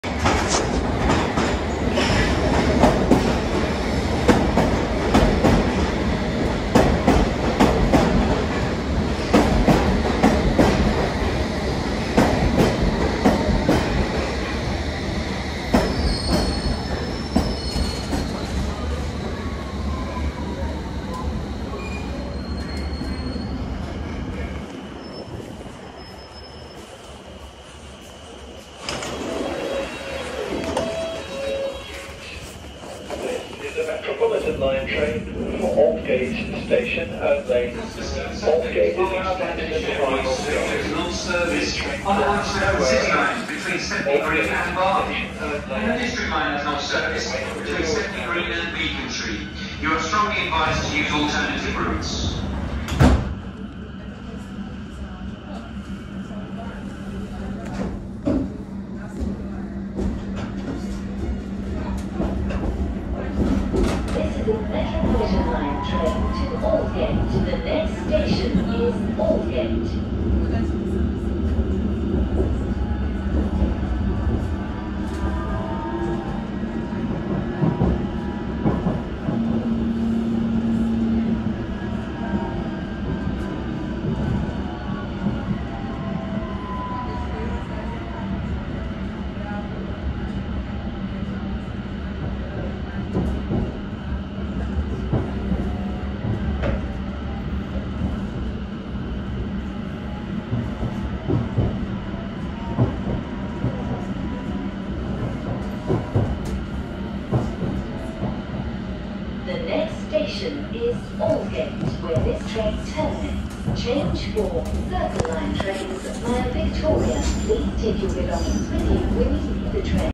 Travel chaos\ train cancellations_on the underground_Metropolitan line.london tfl #2023|| January
Join me on a first-person journey as we take a ride on a metropolitan train. Experience the hustle and bustle of city life as we pass by iconic landmarks and bustling streets. From the comfort of our train, watch the world go by and see the diverse neighborhoods that make up the city. Get a glimpse of the daily commute for locals and see what it's like to navigate the busy train system. This is a must-see for anyone visiting the city or just looking for a unique perspective on urban life.